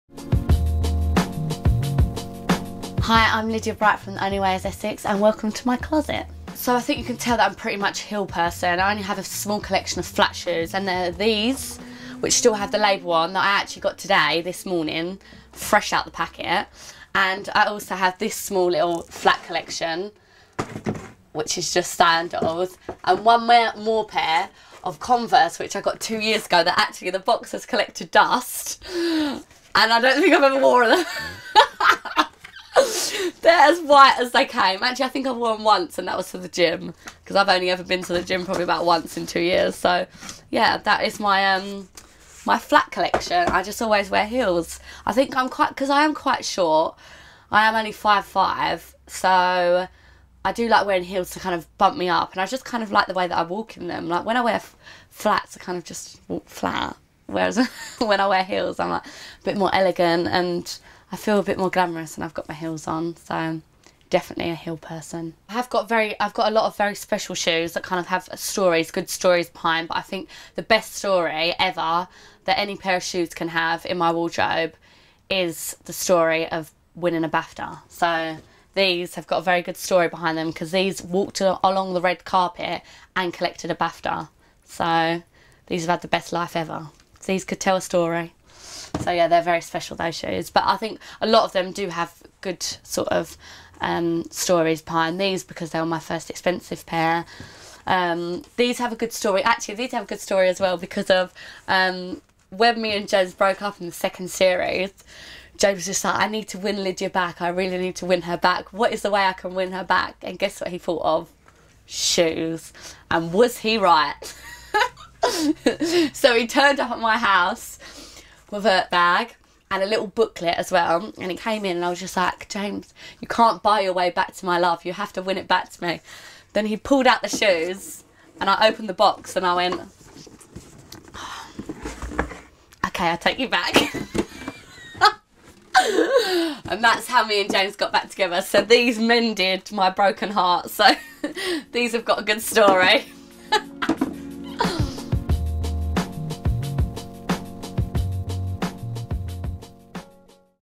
Javari.co.uk Shoe the Keyhole Series: Lydia Bright
Javari.co.uk takes a trip to Essex for a sneaky peek into the wardrobe of 'The Only Way is Essex' star Lydia Bright. Lydia talks to Javari.co.uk about her favourite pairs of shoes, why she owns a rainbow selection of the same style and the unforgettable time Arg tried to woo her with Louboutins that were too small...

For all you shoe fanatics out there 'Shoe the Keyhole' is your chance to peek into the wardrobes of fashion's finest as they spill their best-kept shoe secrets.